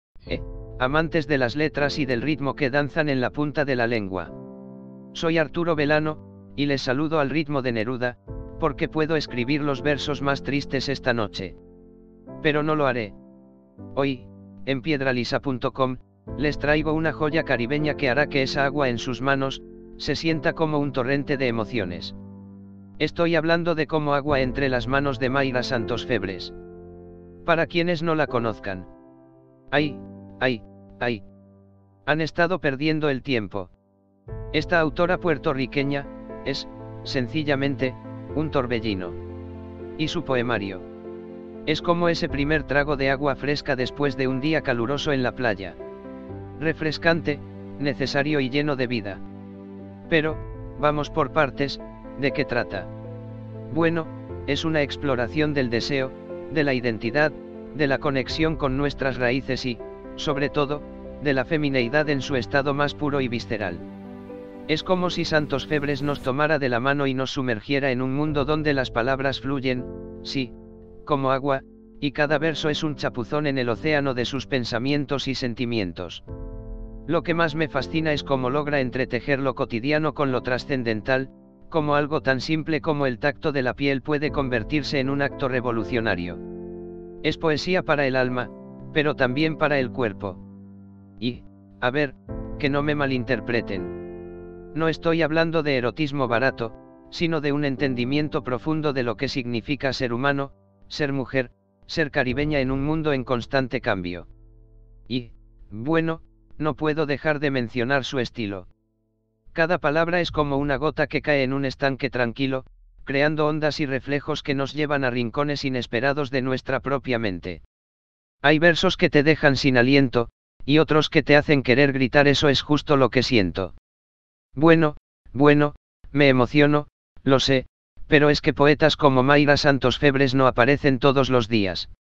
"Como agua entre las manos" de Mayra Santos-Febres: Un Chapuzón en la Poesía Caribeña
Sumérgete con Arturo Belano en el vibrante poemario "Como agua entre las manos" de la puertorriqueña Mayra Santos-Febres. En este episodio, exploramos la danza entre deseo e identidad, la pasión de ser mujer y caribeña, y la magia de la palabra que fluye como agua. Si estás buscando una refrescante ola de poesía contemporánea, ¡este episodio es para ti! 🌊📖